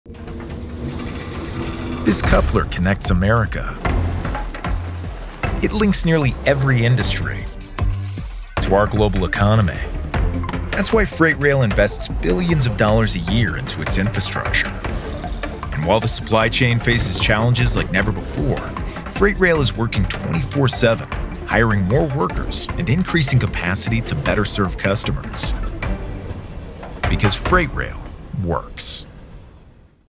Freight Railroads Connect America
Freight railroads serve nearly every industry in America, providing critical connections across the economy. The industry invests well over $20 billion a year into its network to bring U.S. companies and global businesses together. Railroads are operating around the clock to solve supply chain disruptions – hiring more workers, increasing capacity and enhancing coordination to better serve customers. Freight rail is always working to provide a safe, cost-effective and sustainable transportation network to keep America moving.

Sign up for our newsletter at AAR.org/The-Signal